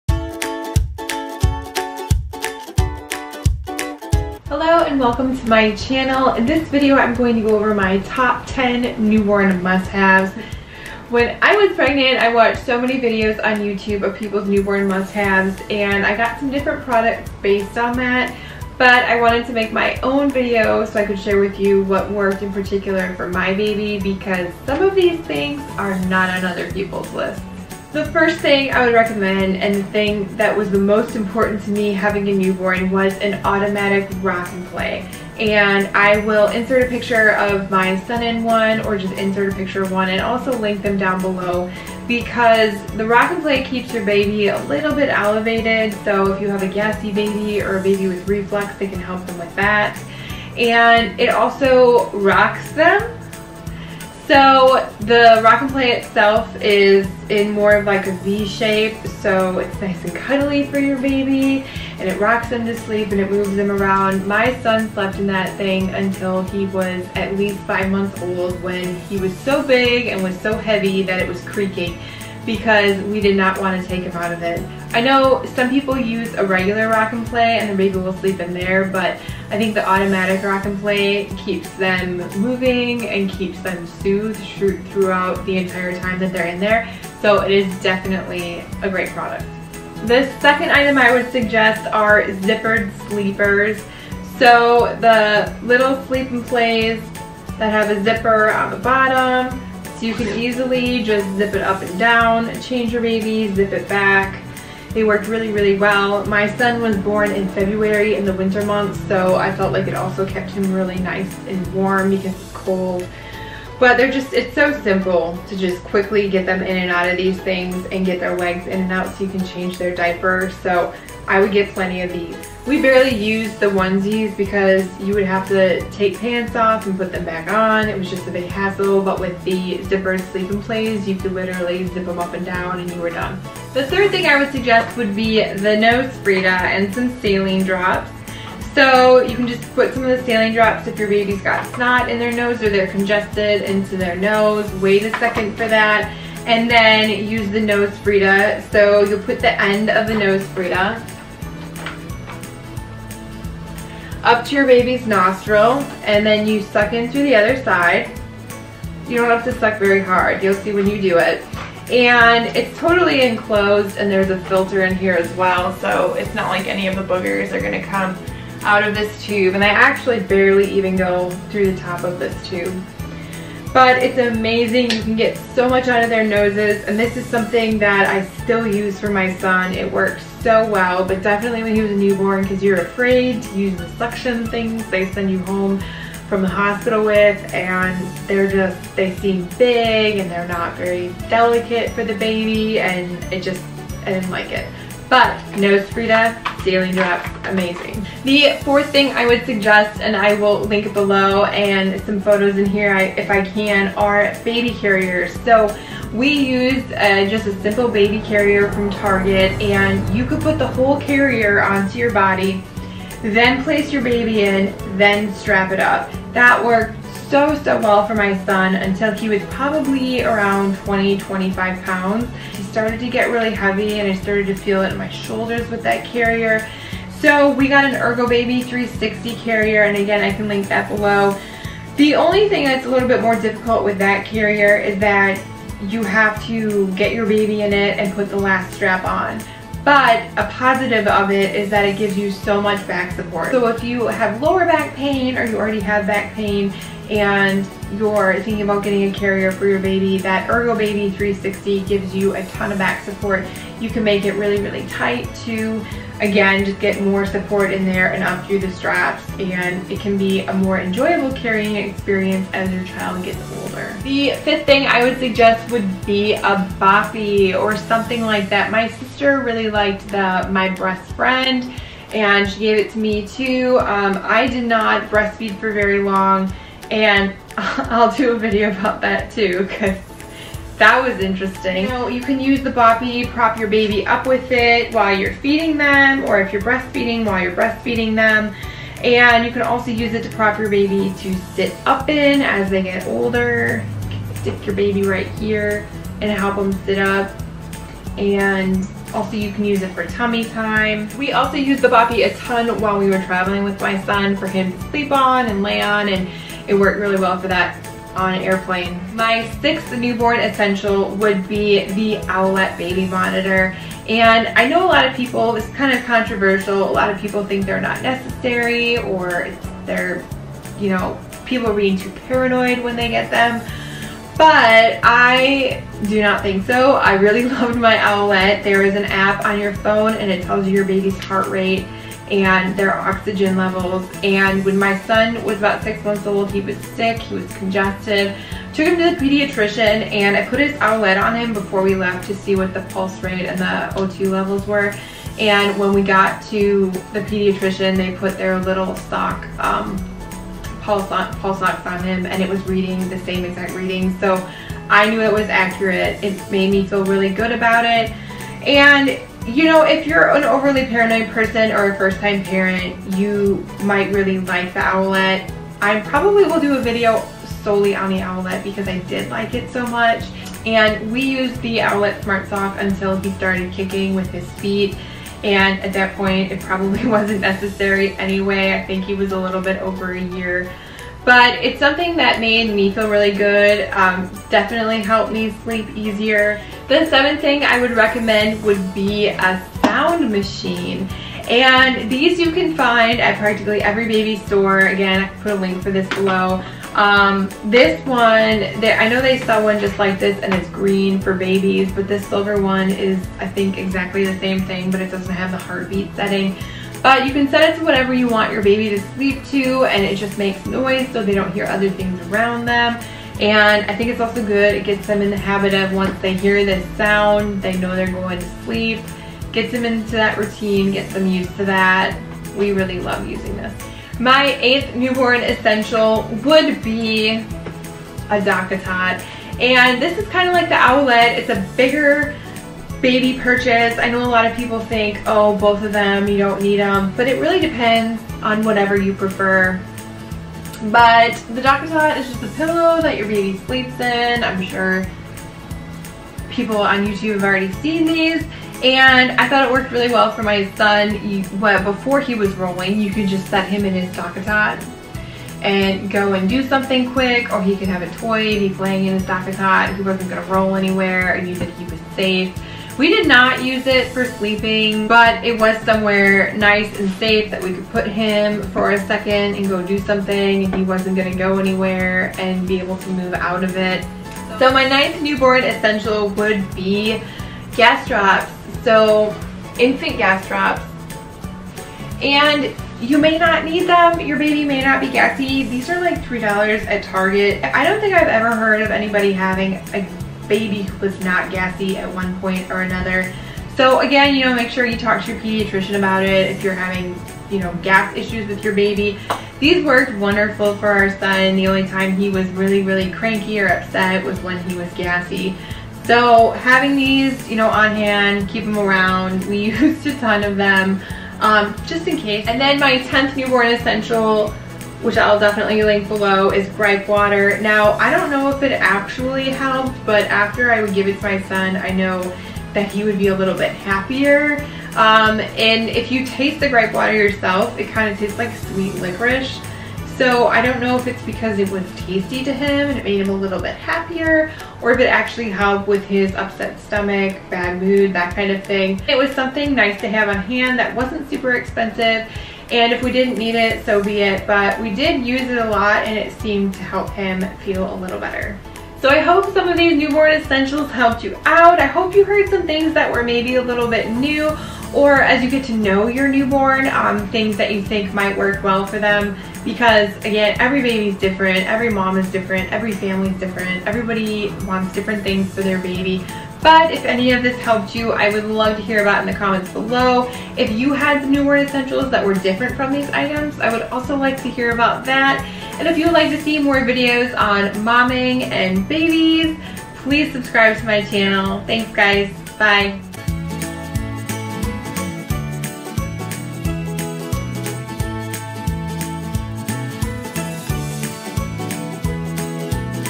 Newborn Must Haves // Newborn Essentials 2018 // Melissa Marie
Newborn essentials and must haves are different for every baby and mama, but some baby products are amazing and so helpful!  In this video, I discuss my top ten newborn essentials.  Below are links to the items I discussed in this video.  I hope this can help some of you mamas out preparing for your little one!

Sleep and Plays

Nose Frida

Baby Carrier

Owlet Smart Sock 2

Sound Machine

Infant Gas Drops

Gripe Water

I am brand new to Youtube, so check back for more videos on motherhood, infants, toddlers, and baby products!

melissaandrhett